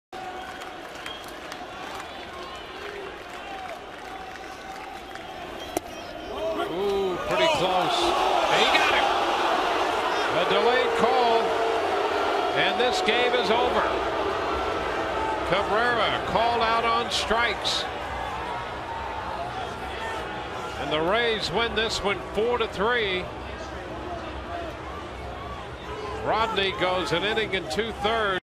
2013/05/21 Rodney slams the door
TB@TOR: Rodney strikes out Melky to earn ninth save